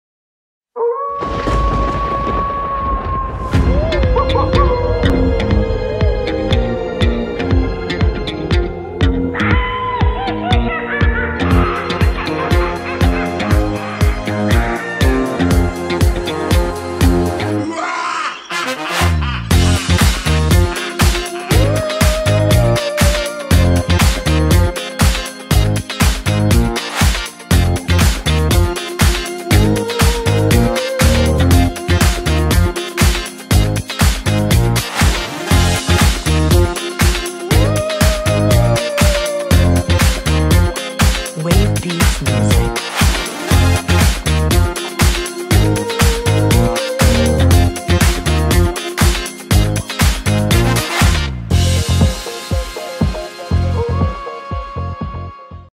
Harbor Terrace Halloween Dance Minute!
A quick dance party for Halloween to get the residents going! What a blast! :)